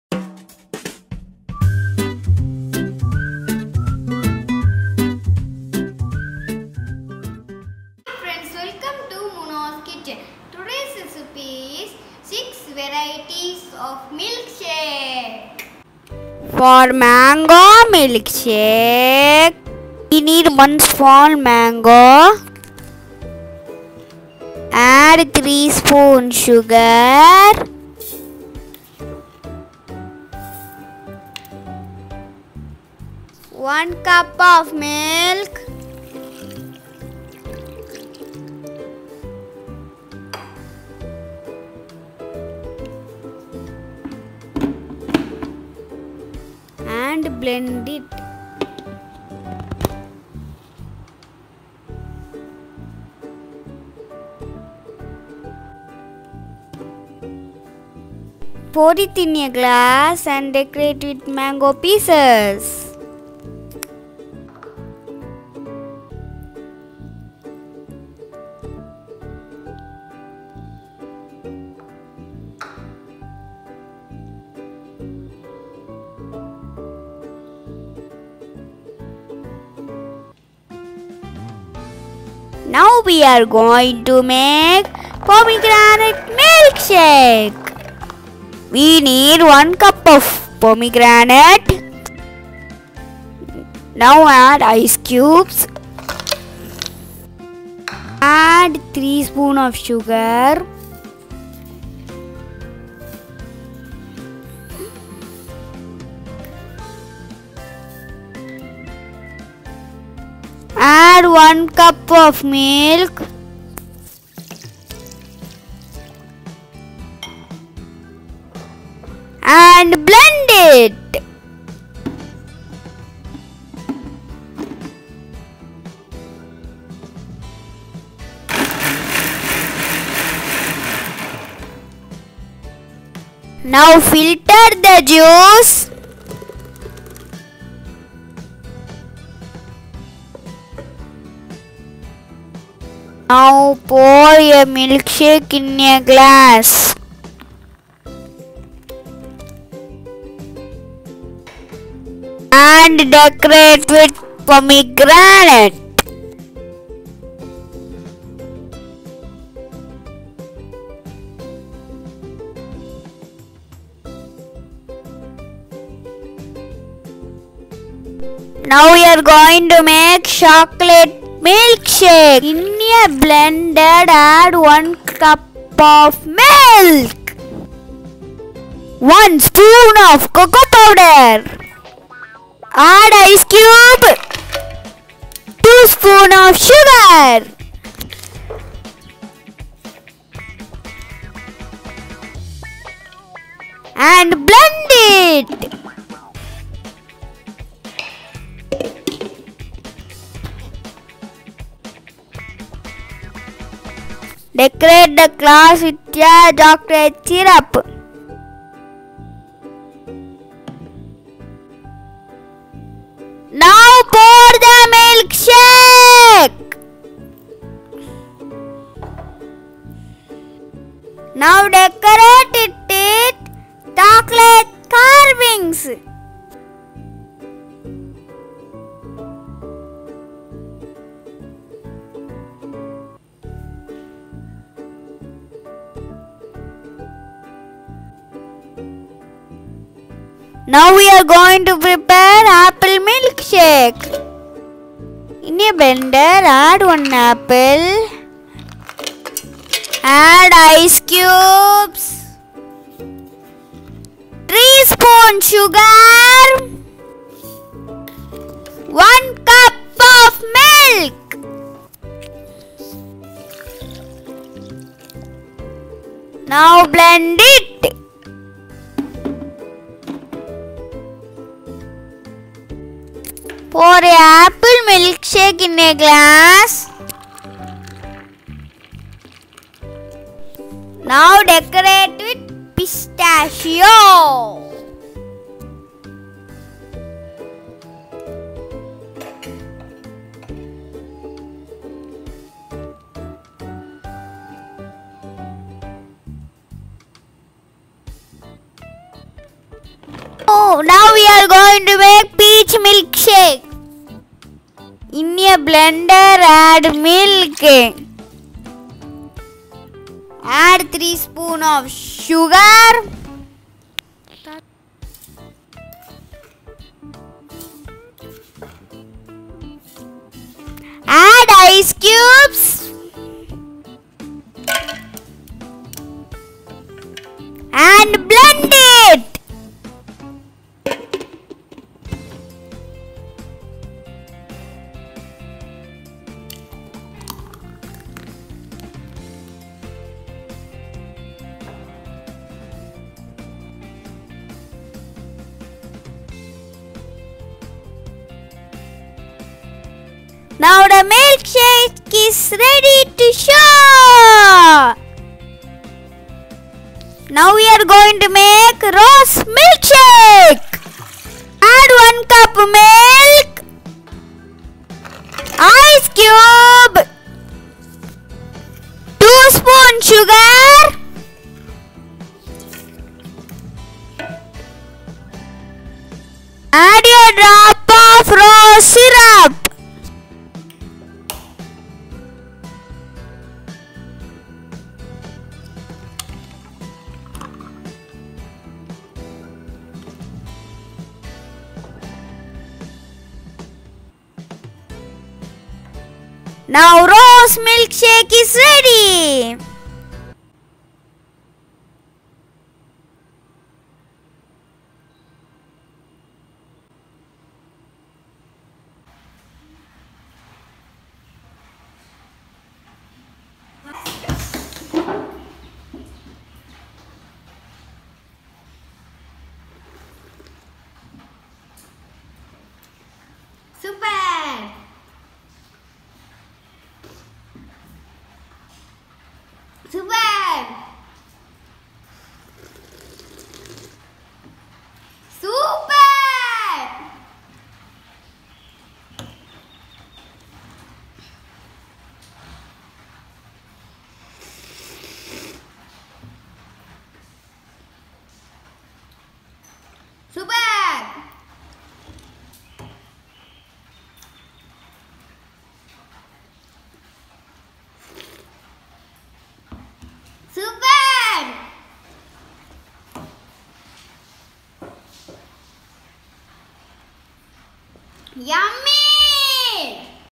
Beat the heat with six varieties of milkshakes(Its so easy!!!!)
There are so much advertisements in tv says us to drink this eat that to restore our energy.But we always recommend to do it in a natural way.Try our six varieties of milkshakes which irresistibly good and immensely nutritious which will give us the satisfaction and the energy which we need throughout the long day...